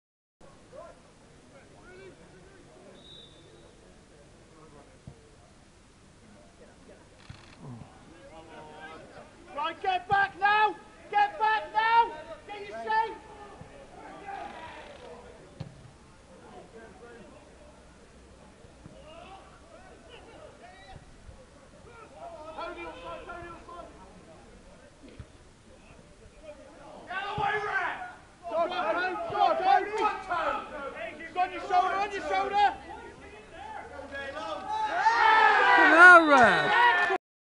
CCFA Cornwall Junior Cup Semi Final Pendeen Rovers 0 - 1 St Stephen
Highlights. Craig Coad is again fouled by Pendeen no 2.